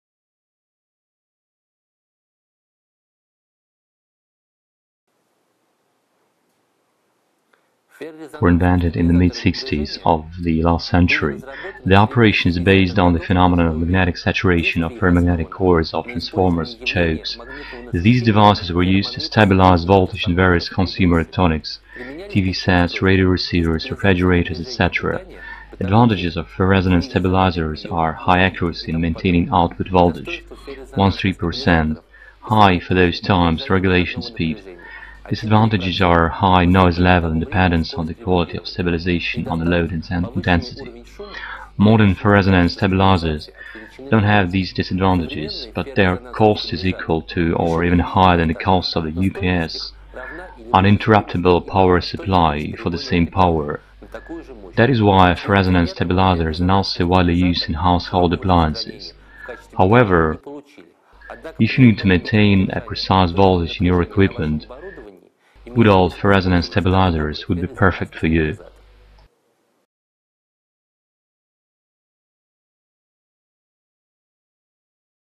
Ferroresonance Voltage Stabilizers
Ferroresonance Voltage StabilizersFerroresonance voltage stabilizers were invented in the mid 60's of the last century. Their operation is based on the phenomenon of magnetic saturation of ferromagnetic cores of transformers or chokes. These devices were used to stabilize voltage in various consumer electronics (TV sets, radio receivers, refrigerators, etc.). Advantages of ferroresonance stabilizers: high accuracy in maintaining output voltage (1-3%), high (for those times) regulation speed. Disadvantages: high noise level and dependence of the quality of stabilization on the load intensity. Modern ferroresonance stabilizers don't have these disadvantages, but their cost is equal to or even higher than the cost of a UPS (uninterruptible power supply) for the same power. That is why ferroresonance stabilizers are not so widely used in household appliances. However, if you need to maintain a precise voltage in your equipment, good old ferroresonance stabilizers would be perfect for you.